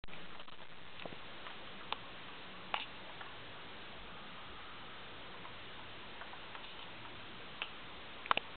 Snow on 12-22-2010
Snowing in Flagstaff, AZ. A couple of hours later the back yard was filled with elk.